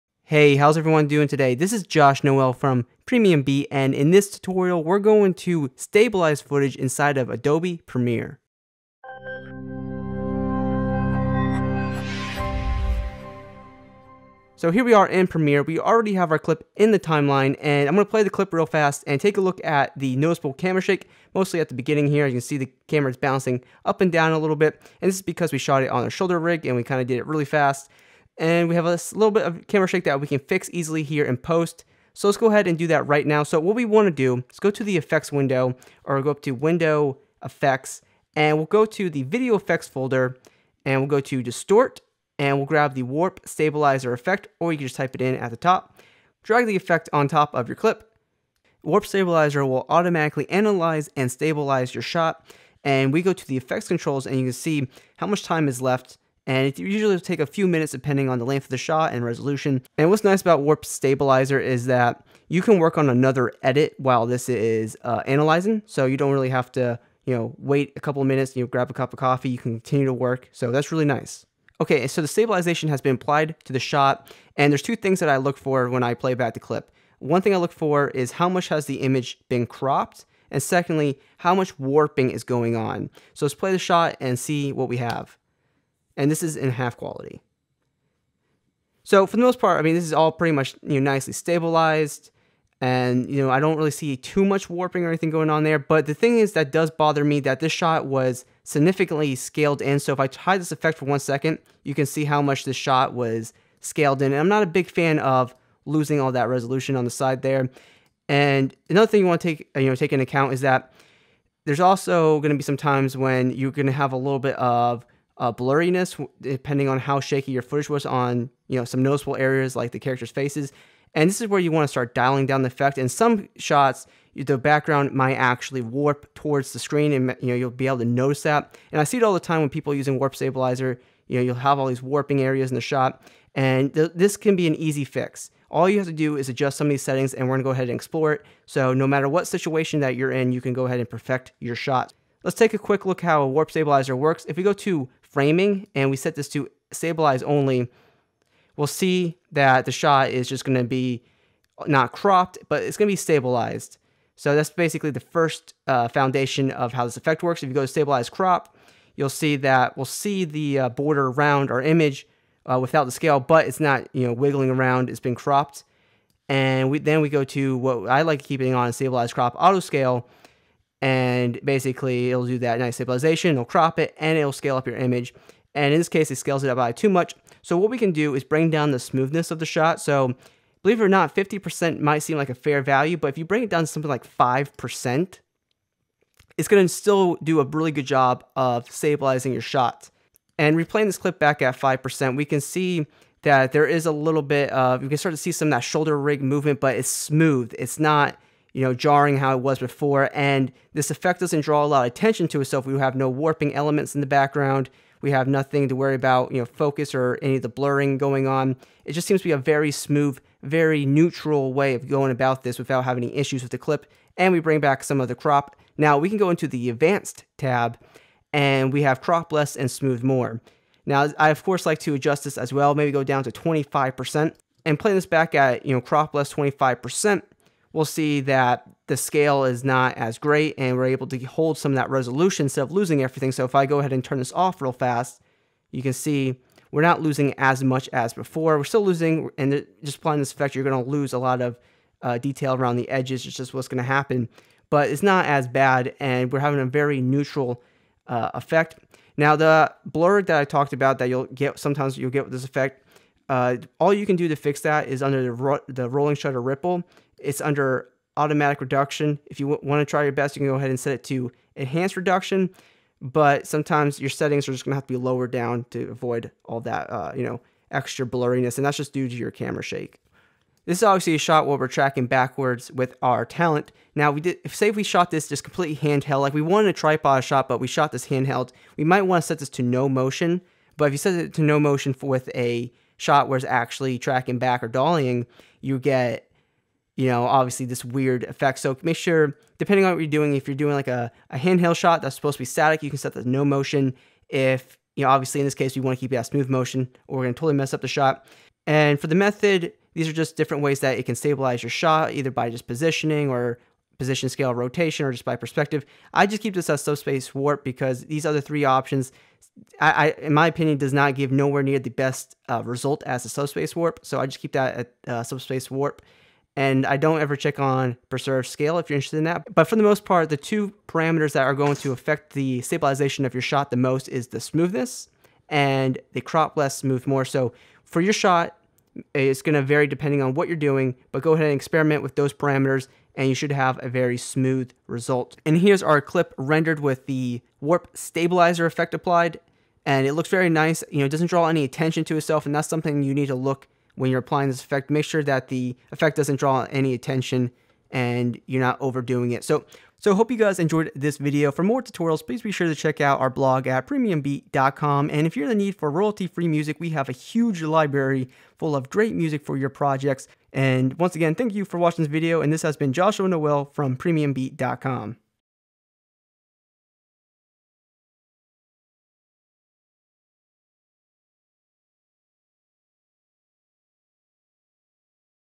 Using Warp Stabilizer to Fix Shaky Footage in Premiere Pro | PremiumBeat.com
- Read more and follow along with step-by-step instructions on using the Warp Stabilizer effect in Premiere Pro on the PremiumBeat blog.